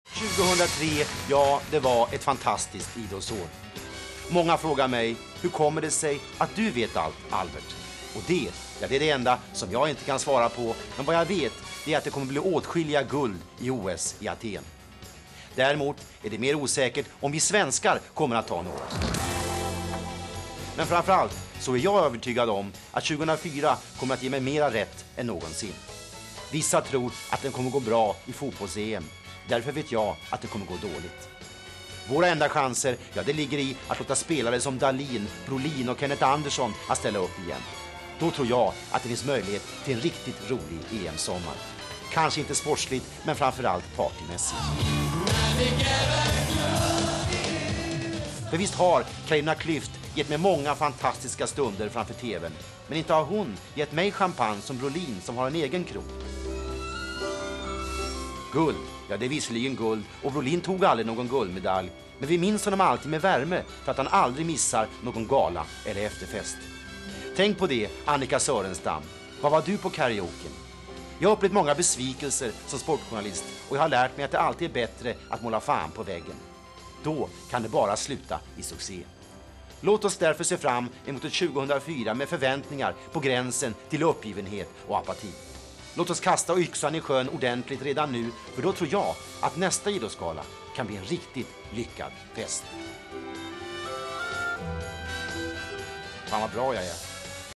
Albert Svanberg - Sportkrönika (Robert Gustafsson)
Albert Svanberg summerar idrottsåret 2003 med en krönika.